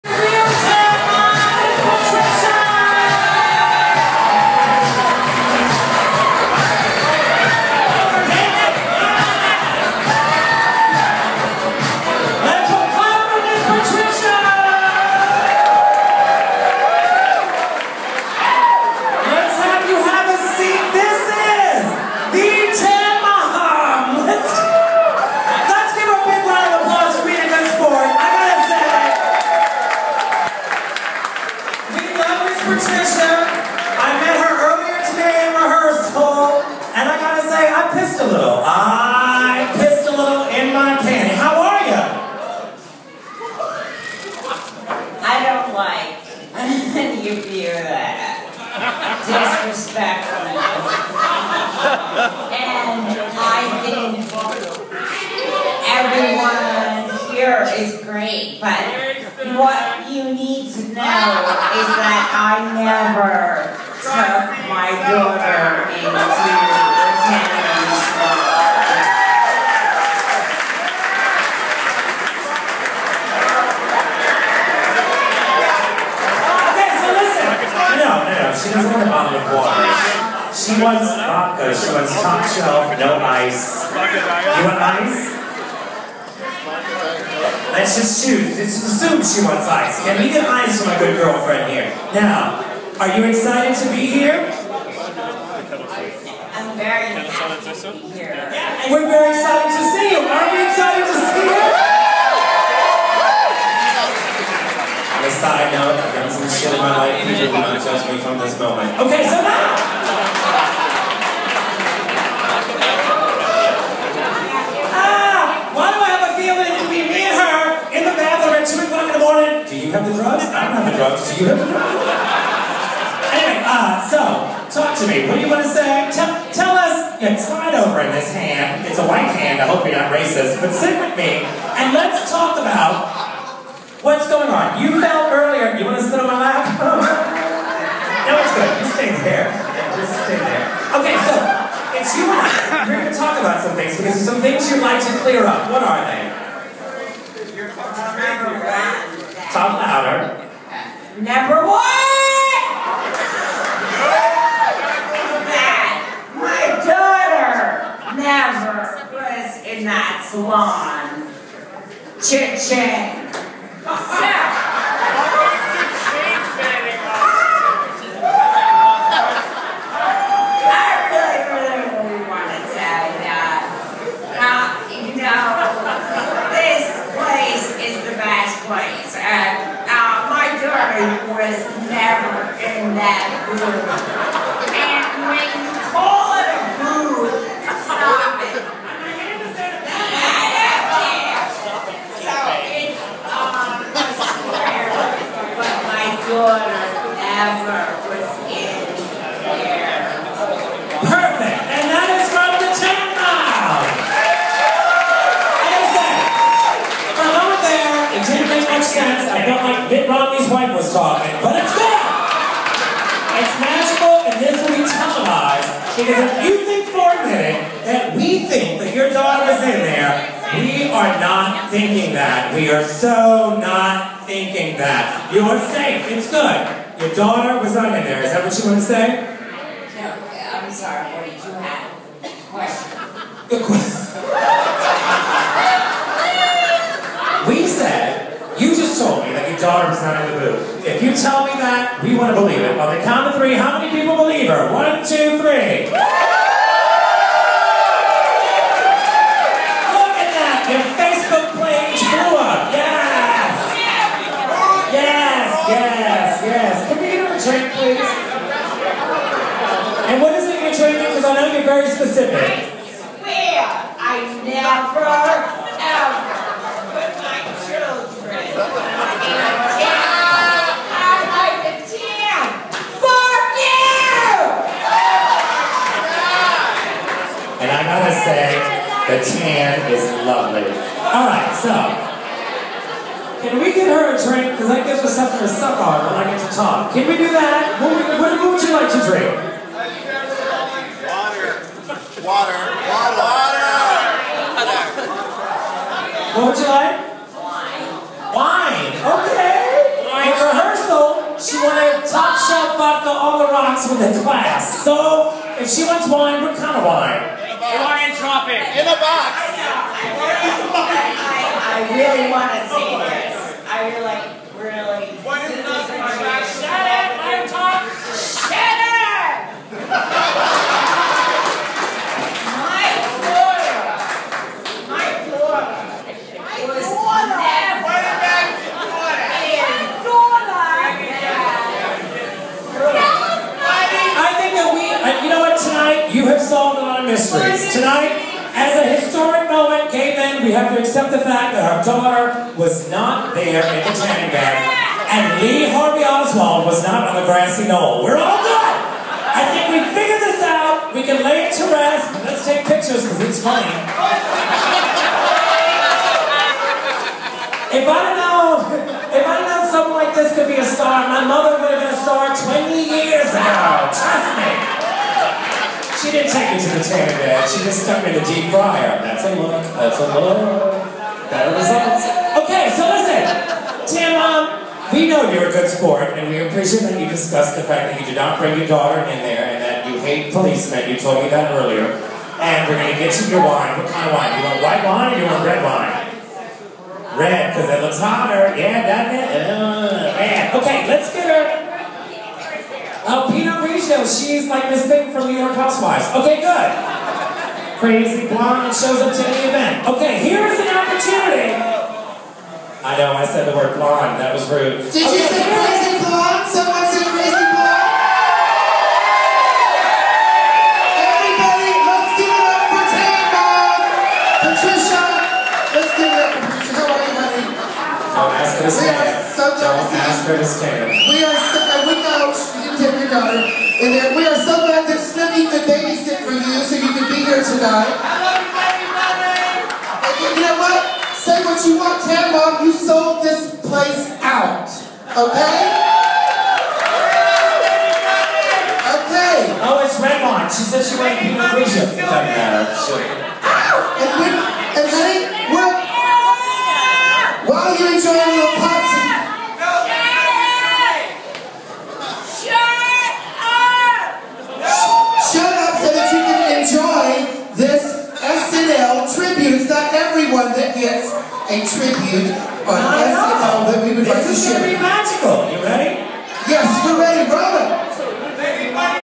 Tan Mom at HOT MESS Wednesdays at XL Nightclub
Tan Mom (Patricia Krentcil) appeared LIVE at Brandon Voss, Lady Bunny and Bianca Del Rio's weekly HOT MESS Drag Review at XL Nightclub & Cabaret. Showtime every Wednesday at 9:30.